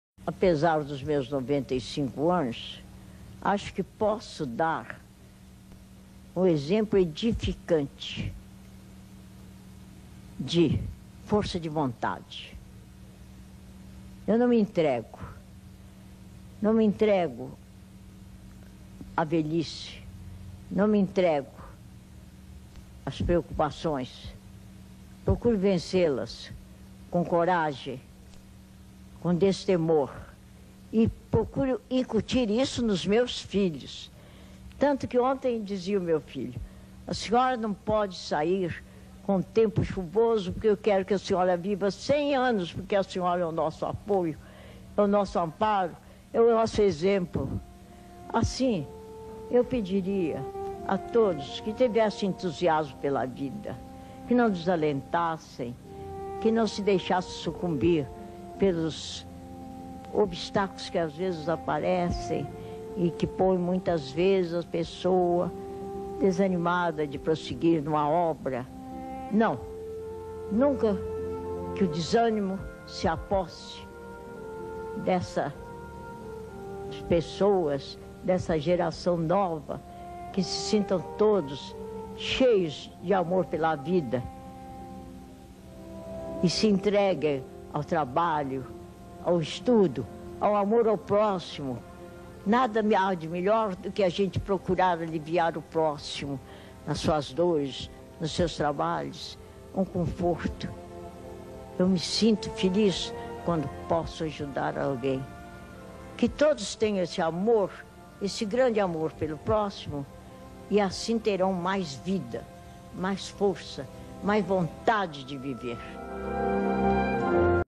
Dona Celina Viegas Vídeo Motivacional
CELINA VIEGAS, professora e historiadora de São João Del Rei, Minas Gerais, faleceu em 2000 com 109 anos. Neste vídeo ela tinha 95 anos quando concedeu entrevista à TV Bandeirantes e deixou seu recado aos jovens pedindo que sejam pessoas mais entusiásticas. Vale destacar que ENTUSIAMO etimologicamente é uma palavra de origem grega formada por in= dentro + theos = Deus, quer dizer, DEUS DENTRO DE SI, ser detentor do tônus vital, de vida que é energia.

"Eu, com meus 95 anos, posso dar um exemplo edificante de força de vontade. Eu não me entrego, não me entrego à velhice, não me entrego às preocupações. Procuro vencê-las com coragem, com destemor e procuro incutir isso nos meus filhos. Tanto que ontem dizia meu filho: 'A senhora não pode sair com o tempo chuvoso, porque eu quero que a senhora viva 100 anos, porque a senhora é o nosso apoio, é o nosso amparo, é o nosso exemplo'. Assim, eu pediria a todos que tivessem entusiasmo pela vida, que não se deixassem sucumbir pelos obstáculos que, às vezes, aparecem e que desanimam pessoas de prosseguir numa obra. Não, nunca deixem que o desânimo se aposse dessas pessoas, dessa geração nova, que se sintam todos cheios de amor pela vida e se entreguem ao trabalho, ao estudo, ao amor e ao próximo. Nada há de melhor do que nós procurarmos aliviar o próximo das suas dores, de seus trabalhos oferecendo-lhe conforto. Eu me sinto feliz quando posso ajudar alguém. Que todos tenham esse amor, esse grande amor pelo próximo, e assim terão mais vida, mais força, mais vontade de viver".